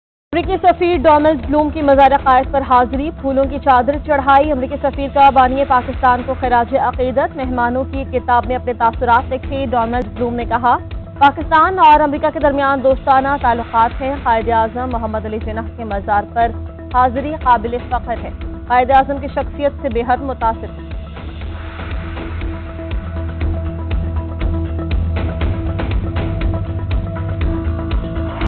Donald Blome a U.S. Ambassador to Pakistan pays respect to Quaid e Azam Muhammad Ali Jinnah GNN
KARACHI – US Ambassador Donald Blome visited the mausoleum of Quaid-e-Azam Muhammad Ali Jinnah and paid his respects to the founder of Pakistan ahead of country’s 75th Independence Day.
Ambassador Blome laid a floral wreath at the mausoleum and signed the guest book on behalf of the U.S. Embassy in Islamabad.
Ambassador Blome later visited the tombs of Liaquat Ali Khan, Pakistan’s first Prime Minister, and Fatima Jinnah, Jinnah’s sister. He toured the adjoining museum and admired the artifacts related to the life of Quaid-e-Azam Muhammad Ali Jinnah. Ambassador Blome noted in the guestbook, “It is a great honor to be here at the Mazar-e-Quaid to pay my respects and commemorate the legacy of Muhammad Ali Jinnah. United States shares Quaid-e-Azam’s vision of a Pakistan at peace with itself and its neighbors, a Pakistan of religious tolerance, economic prosperity, and social inclusion. On behalf of the American people, I offer Pakistan warm congratulations on its 75th Independence Day.” This year marks 75 years of bilateral relations between the United States and Pakistan. The United States values this long-standing cooperation with Pakistan and has always viewed a strong, prosperous, and democratic Pakistan as critical to U.S. interests.